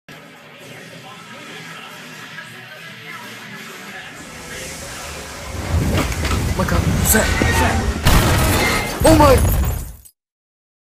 Video FX from iPhone 4S
I made this video using the new iPhone 4s camera and an app called Action Movie FX  Enjoy:)